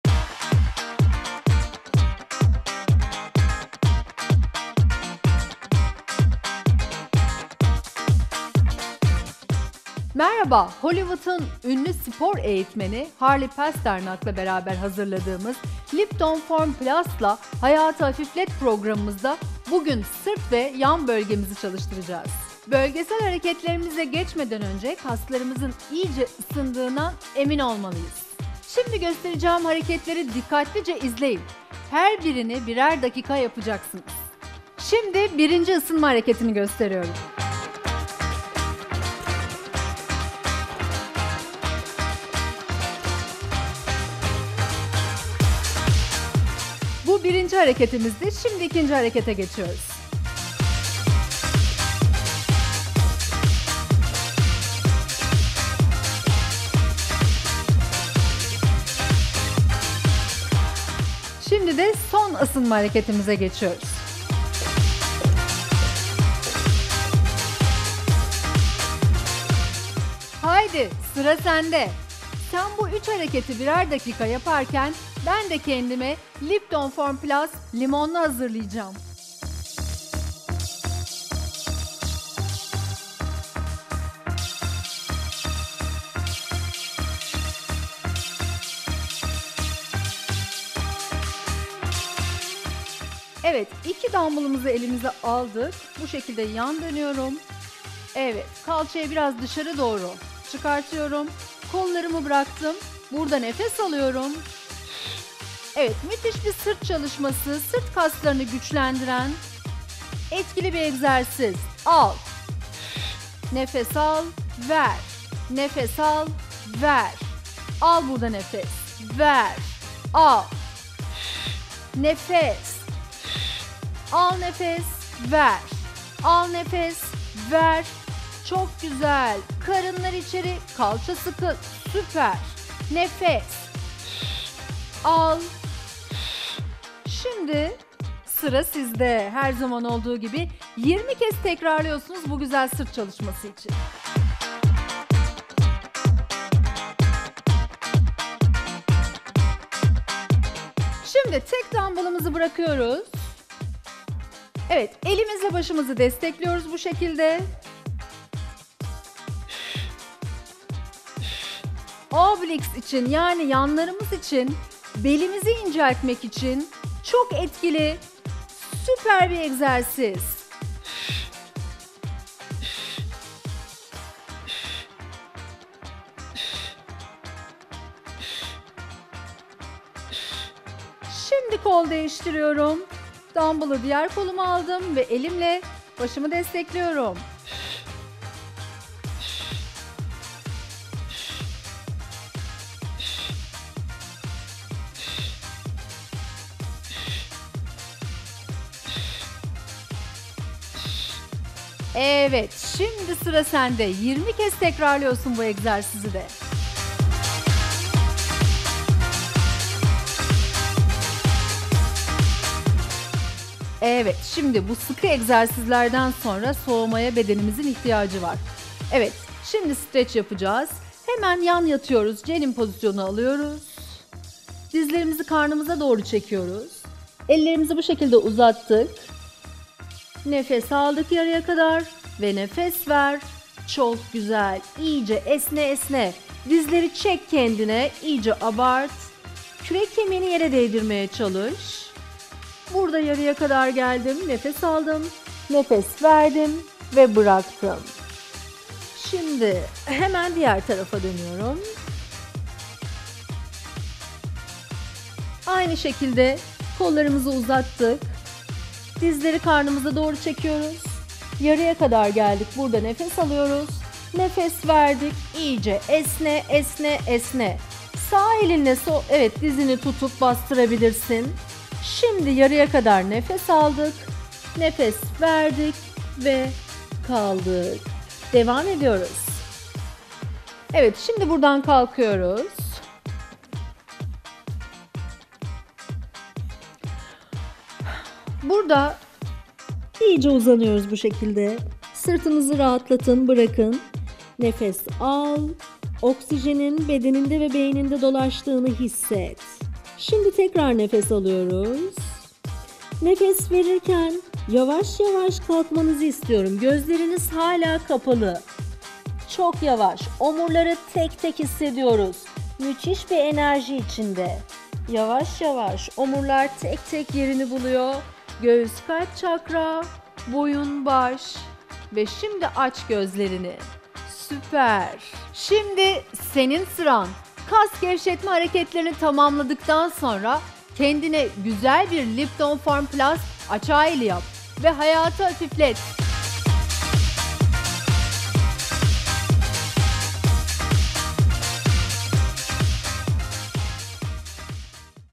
02 - Sırt Bölgesi ve Yanlar
Hem güzelliği hem de formuyla Türk kadınlarına ilham kaynağı olan Ebru Şallı ve Hollywood'un ünlü spor eğitmeni Harley Pasternak ile yaza keyifli bir şekilde hazırlanmak ister misin?